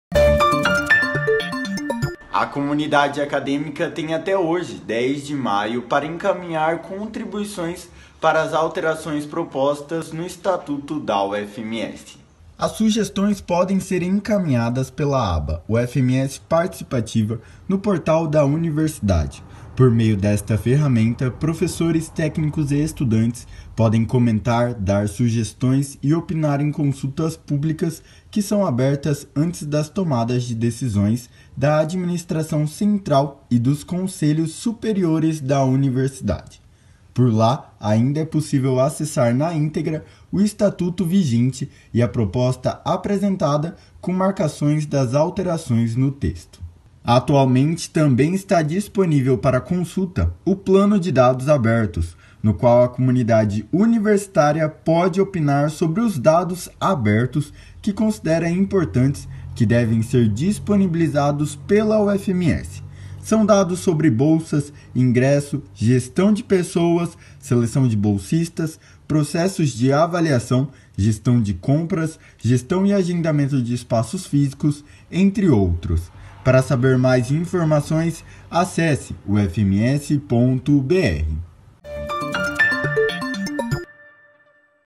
Conexão UFMS - UFMS Participativa
👉🏽 Você sabia que está rolando uma consulta pública na Universidade? E que a comunidade acadêmica pode encaminhar contribuições para as alterações propostas no Estatuto da UFMS? Mas fique ligado! Os encaminhamentos devem ser feitos até hoje!

▶️ Saiba mais no Conexão UFMS!